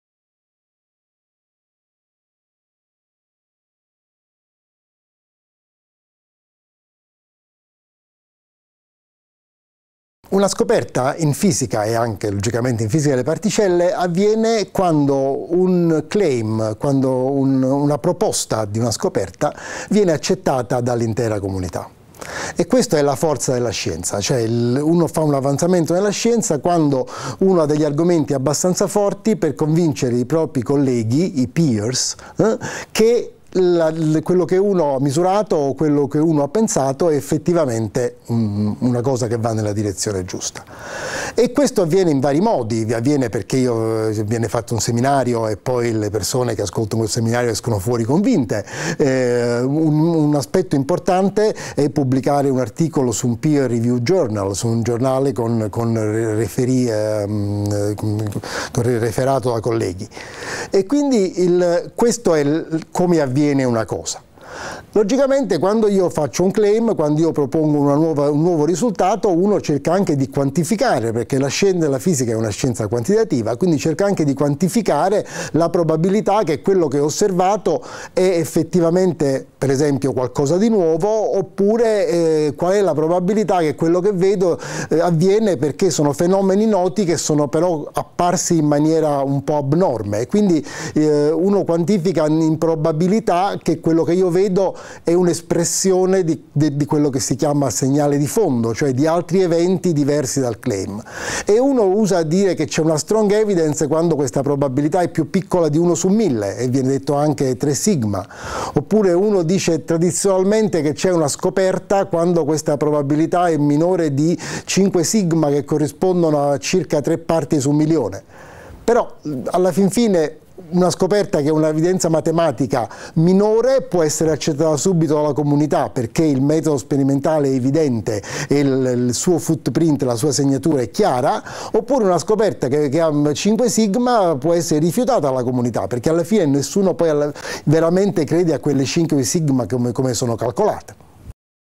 Come si è arrivati alla scoperta del bosone di Higgs?
Come si è arrivati alla scoperta del bosone di Higgs? Perché i risultati di oggi sono più evidenti di quelli del dicembre 2011?
Le risposte a queste domande di un fisico italiano impegnato da anni al CERN di Ginevra, Gigi Rolandi.